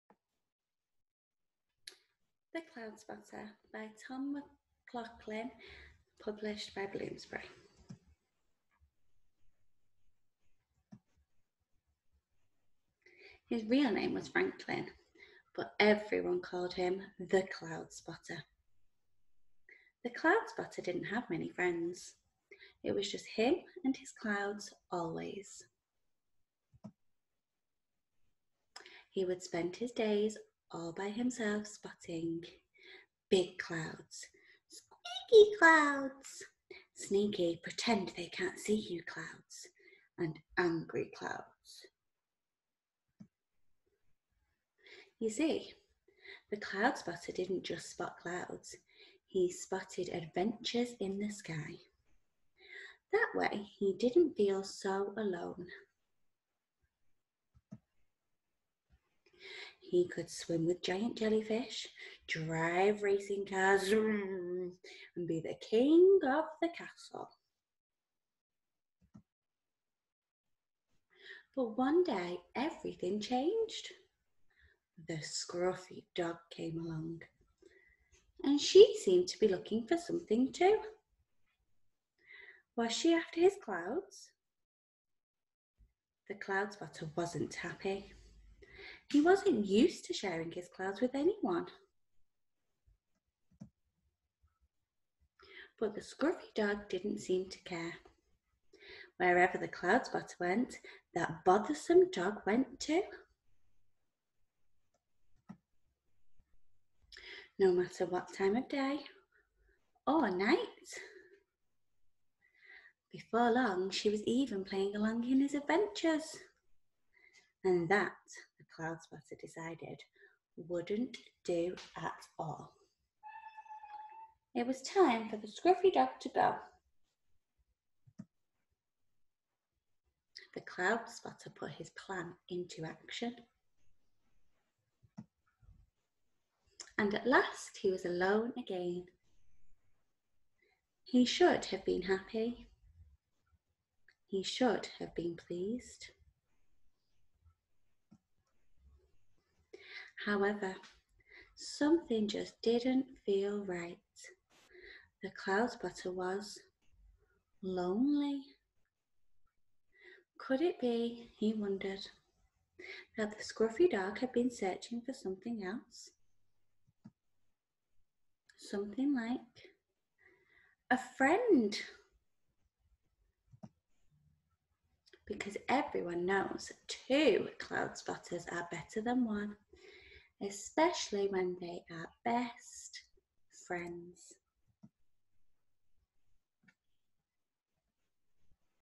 The Cloudspotter
A story about a boy who loves to spot clouds. However, on his adventures he meets someone who isn't quite sure of...